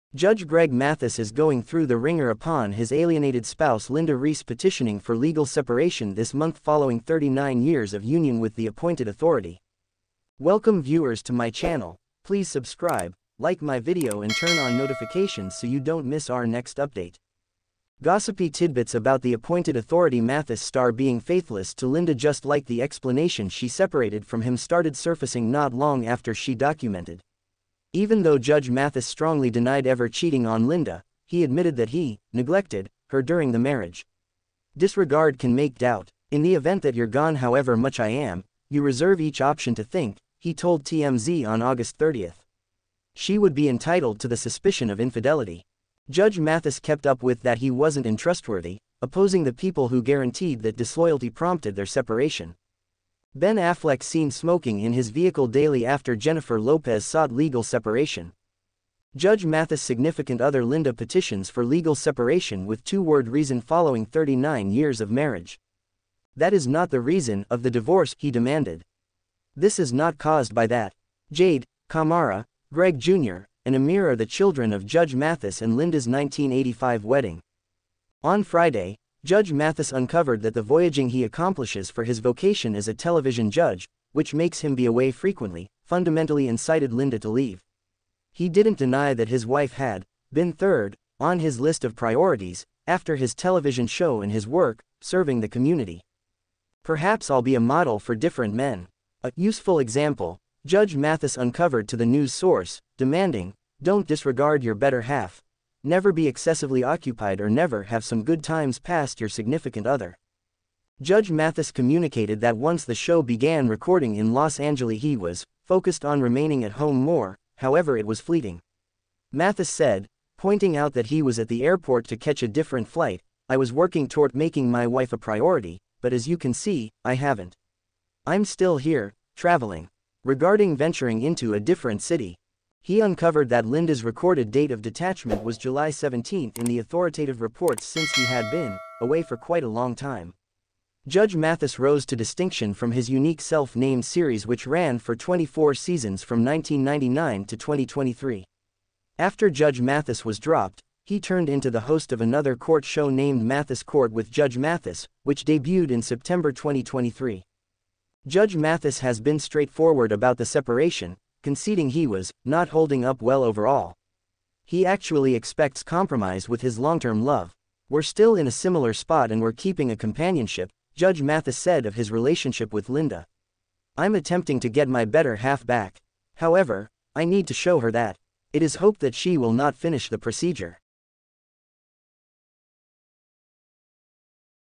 Judge Mathis wife file for divorce after leaked images exposed him with his cheating Mistress kissin
Judge Greg Mathis is going through the wringer upon his alienated spouse Linda Reese petitioning for legal separation this month following 39 years of union with the appointed authority.